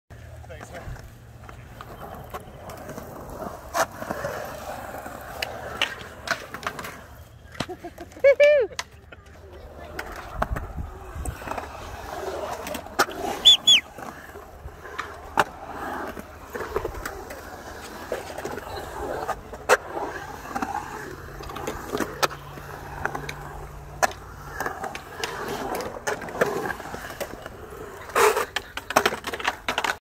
SK8 McVickers in Elsinore 1-31-21.
The drive was nice. Light traffic and the ❄snow on the mountains ⛰looked rad.